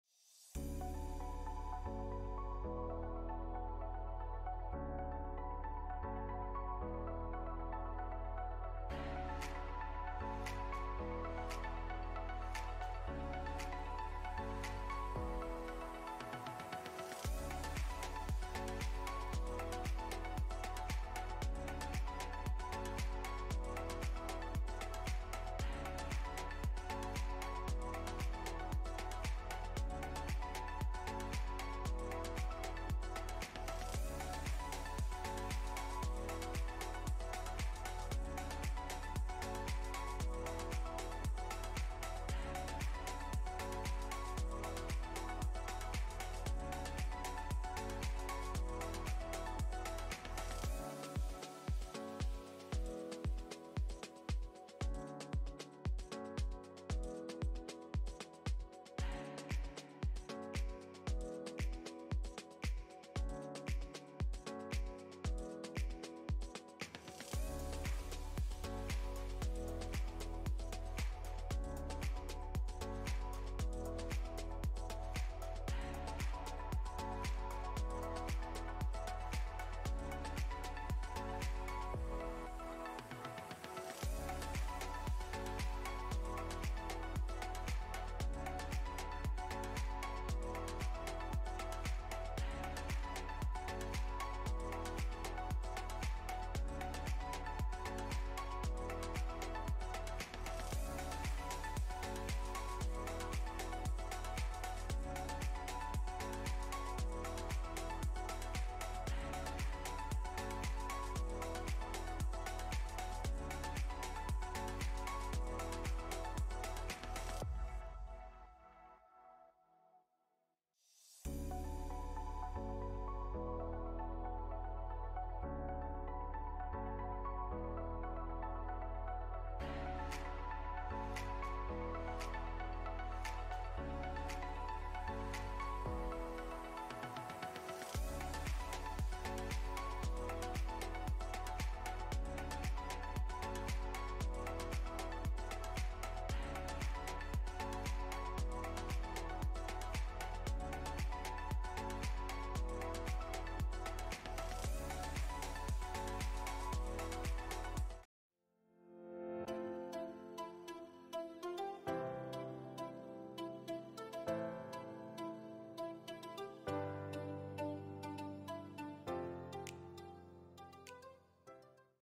【LIVE2D SHOWCASE】Fable Redux
I made some changes, expanded ranges, and added toggles. So here's another showcase for Fable (ver 3.8), in a little bit higher quality.